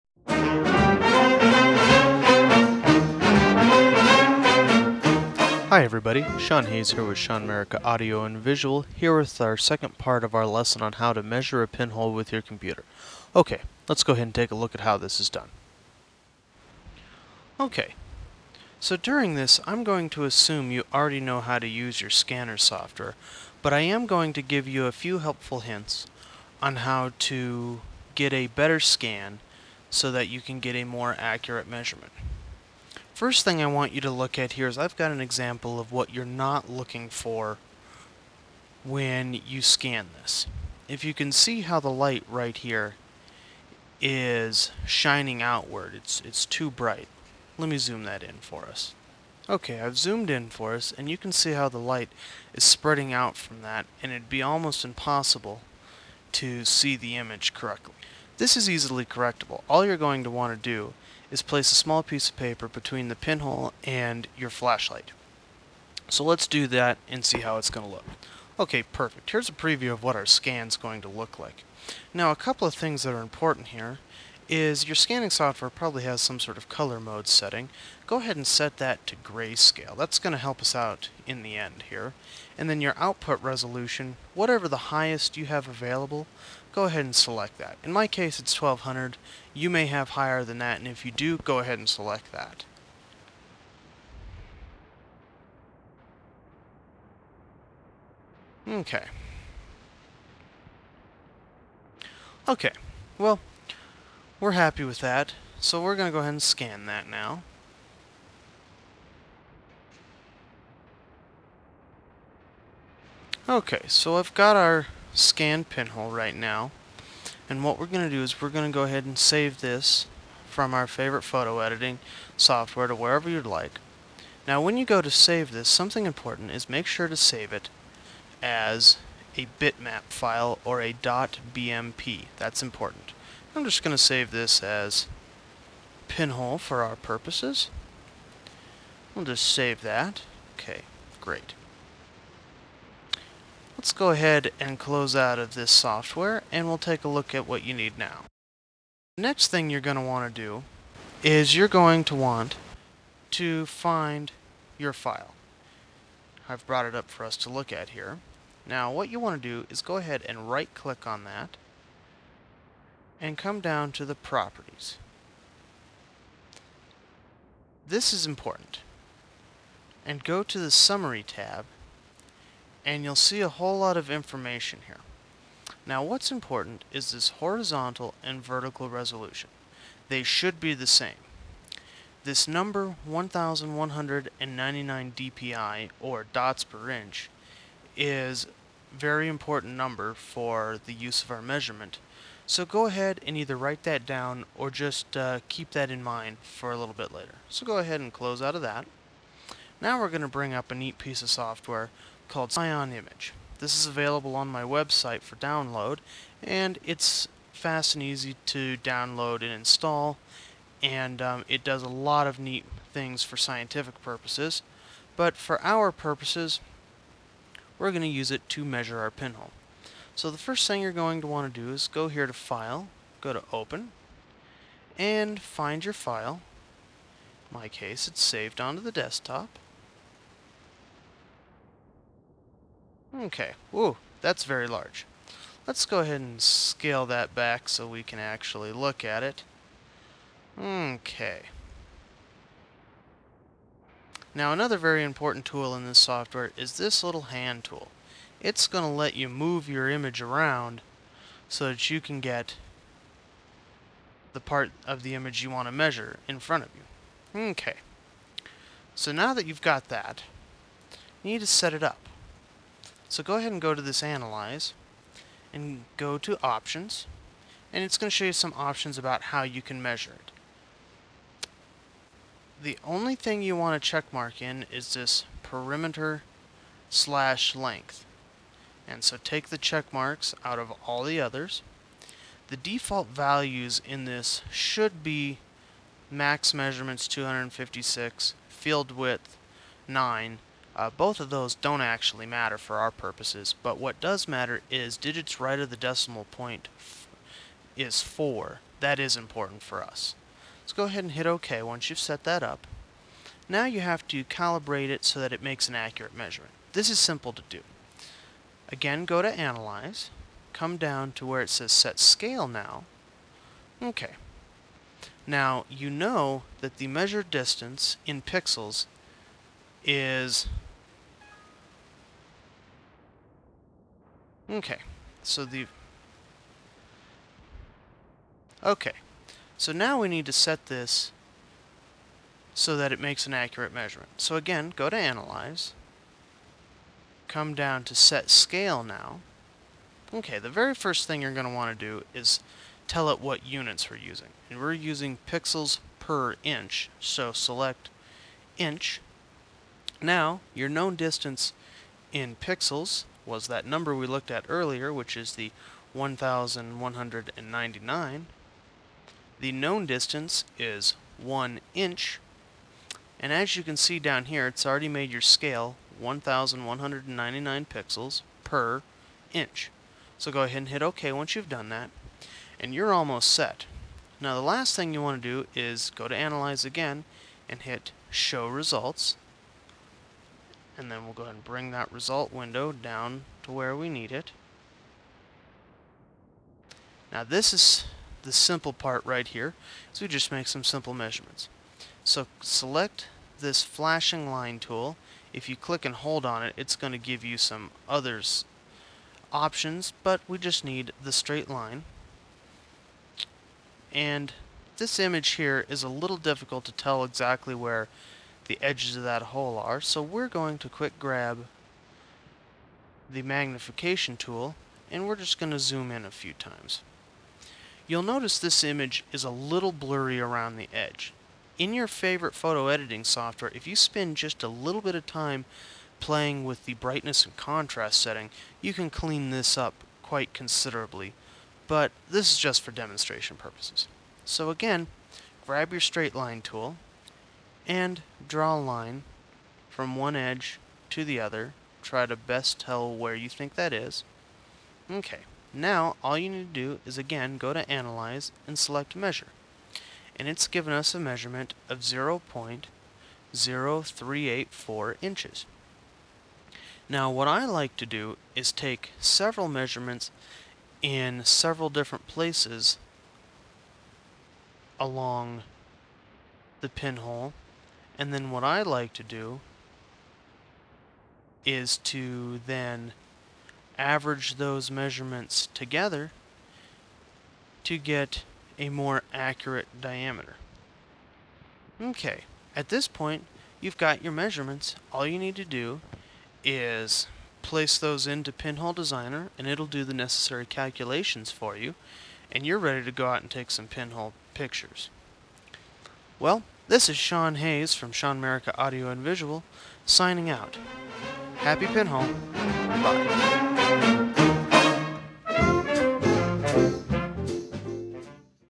How to Pinhole Camera Photography (Lesson 5, 2of 2, Measure a Pinhole) 006
This video lesson is all about how to accurately measure a pinhole, using your computer.  This is 2 of 2. Make sure to visit all of my lessons about pinhole cameras and pinhole photography.